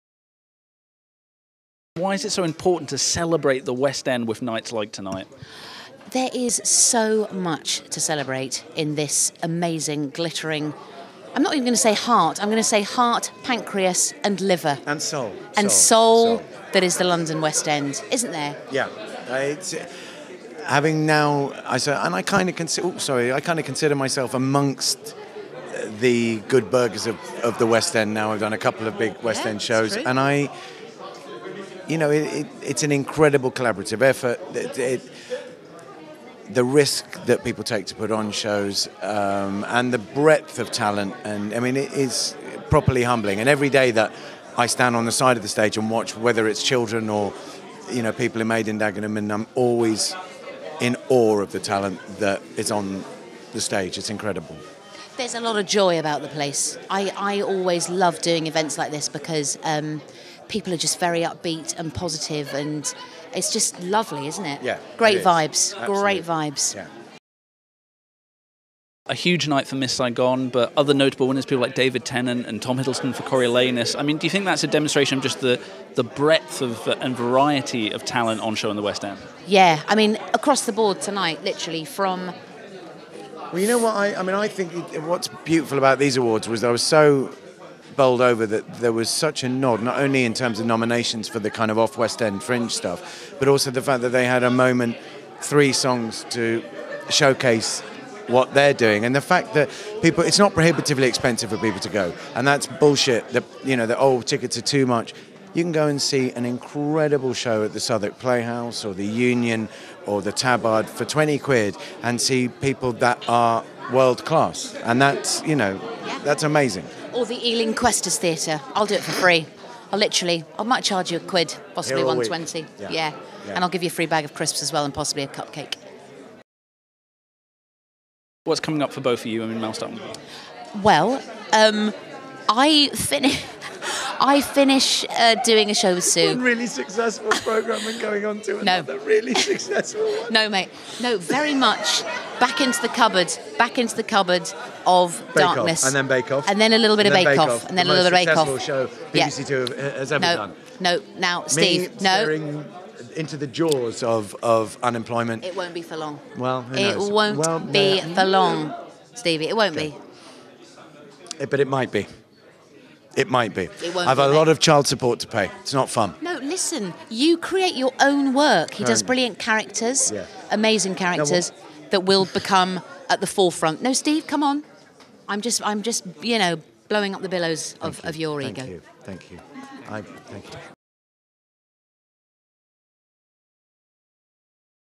Mel Giedroyc & Steven Furst Interview - What's On Stage Awards 2015
Hosts Mel Giedroyc and Steven Furst talk to Red Carpet News backstage at he What's On Stage Theatre Awards in London. This year Miss Saigon, David Tennant & the superb Tom Hiddleston production of Coriolanus were among the winners.